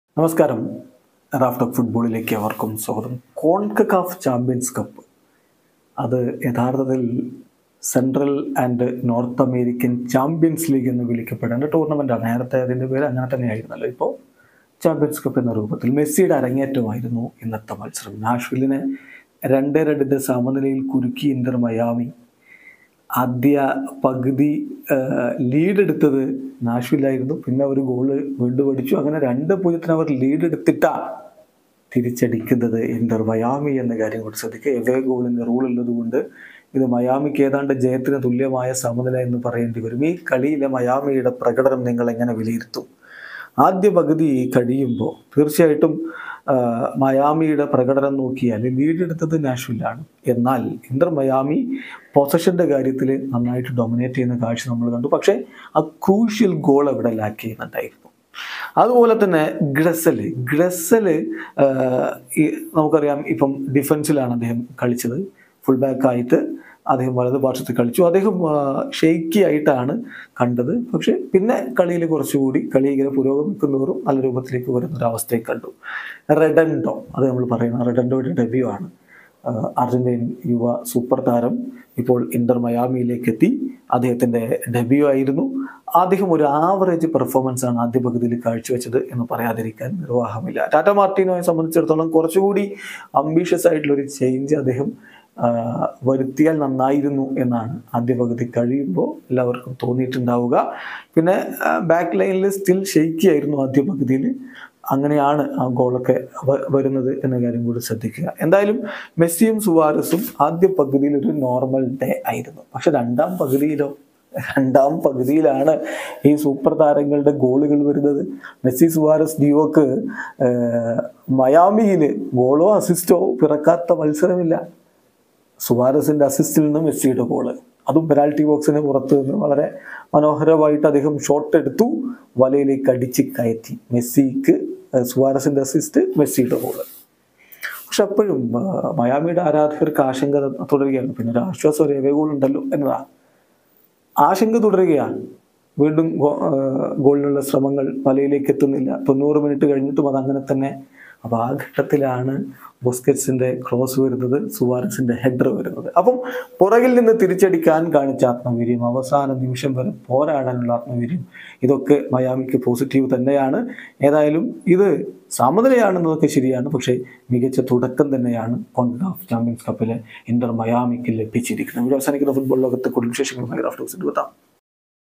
മയാമിയുടെ ഇന്നത്തെ കളി എങ്ങെനെ? പ്രതീക്ഷക്ക് വകയുണ്ടോ...? | Nashville vs Inter Miami
CONCACAF Champions Cup
Nashville vs Inter Miami
Lionel Messi